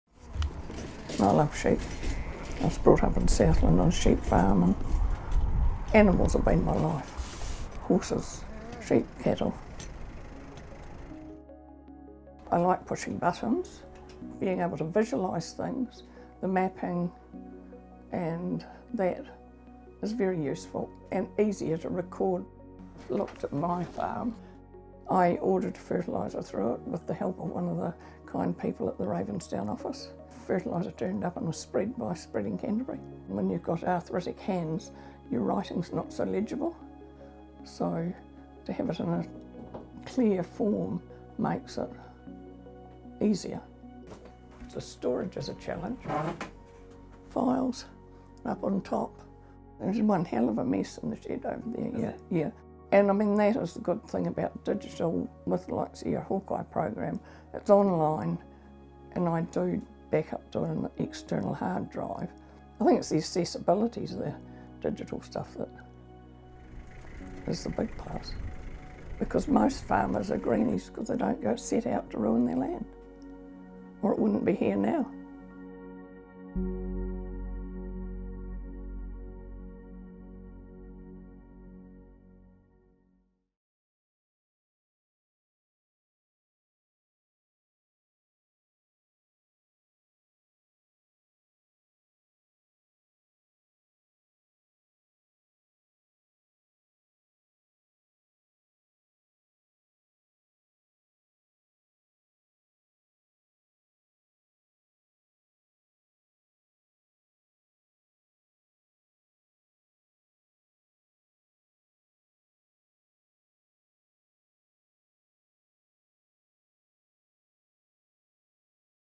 HAWKEYE / AVERIL BULL
Averil uses HawkEye for all her paddock management records. It is much easier for her to type away than write and store bits of paper, especially because all the nutrient information is right there when she needs. Not to mention ordering is made simple, Averil just clicks on a paddock, enters the product, area and rate and the order is spread out before her. You can do the same. Create an account today.